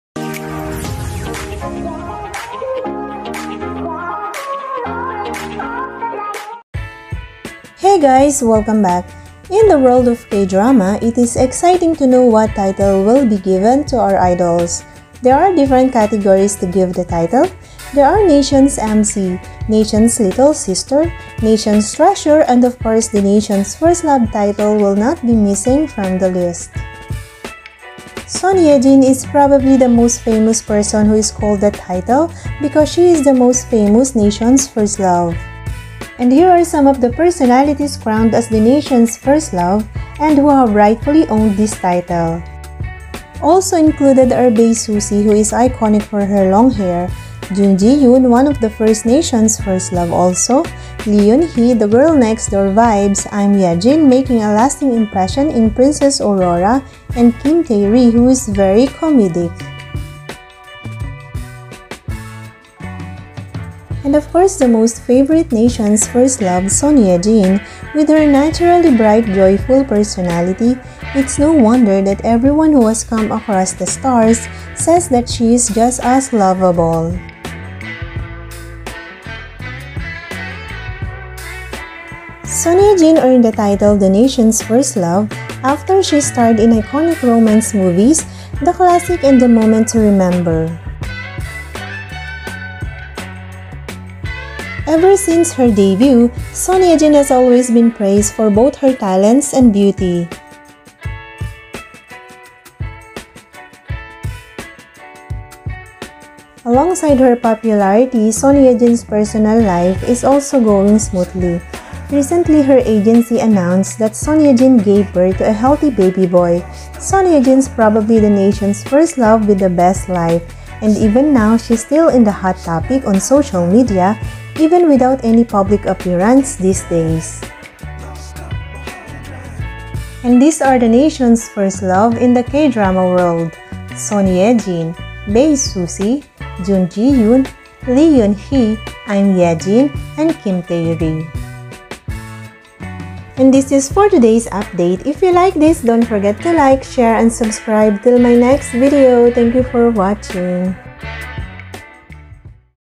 SON YE-JIN RIGHTFULLY OWNED THIS CROWN |CHECK OUT THE FULL LIST OF KSTARS
Sonyejin ranked no. 1 as the Nations first love.
Together with Bae Suzy, Jun Ji Hyun, Lee Yeon Hee, Im Yejin and Kim Tae Ri.

Thanks to all Binjin Fandoms all over the world

Spreading Goodvibes
Godbless 🙏🏻